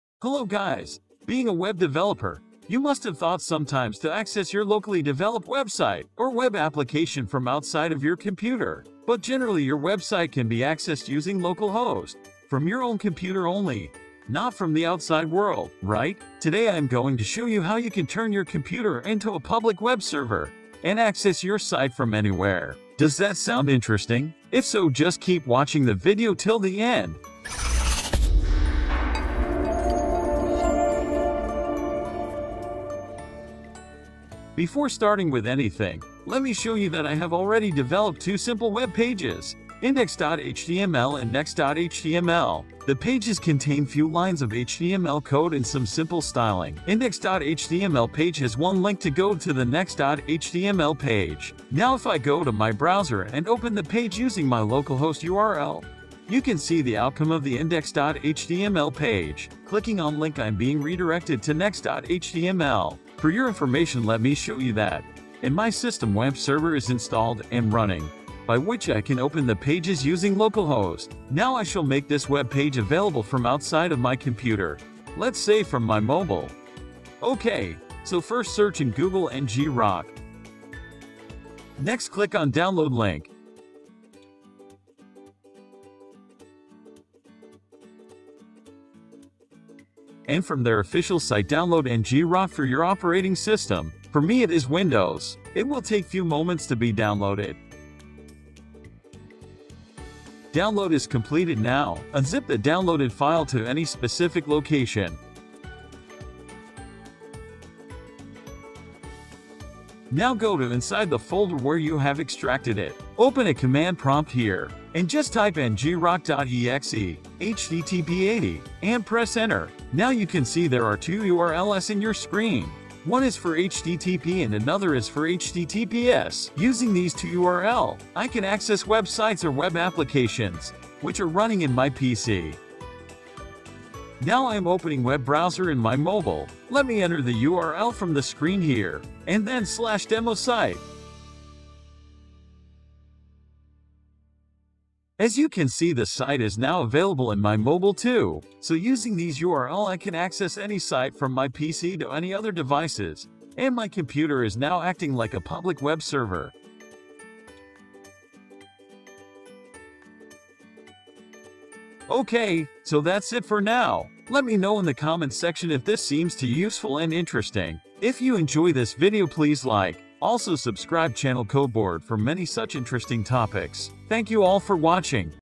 Turn your PC into public server: Make PC to public web server in 2 minutes | NGROK VPN tutorial
Web developers and designers sometimes feel to make their site or application public, may be for some demo or accessing it from some other devices.
Most free hosting sites are not reliable and also most of those does not provide support of other languages apart from PHP; and in some cases they provide static page hosting.

In this scenario we can use NGROK VPN to make site public. It is free to use. You can literally access any web site or web application from your development PC to any other devices. You don't even have to transfer files of your project.

NGROK will make your PC as a public server using VPN service and will allow you to access it via a temporary URL from anywhere. It is a great tool to access you site outside localhost.